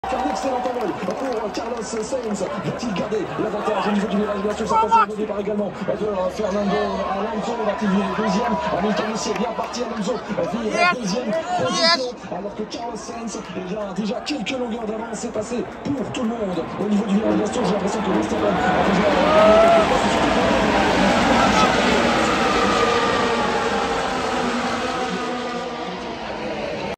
Eau Rouge / Raidillon F1 2022 Gold 3 view at Spa Francorchamps. Max Verstappen wins.
Spa Francorchamps Formula 1 start Gold 3 tribune Eau Rouge.